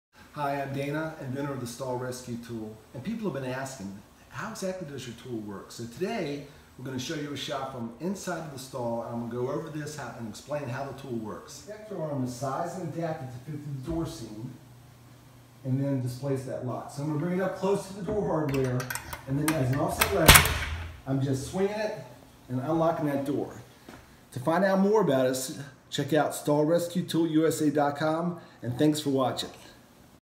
How to use the STALL RESCUE TOOL®
Introducing the STALL RESCUE TOOL®, designed to quickly lock or unlock bathroom stall doors. For use in the event of an emergency, or for janitorial management purposes, this tool is a time-saver!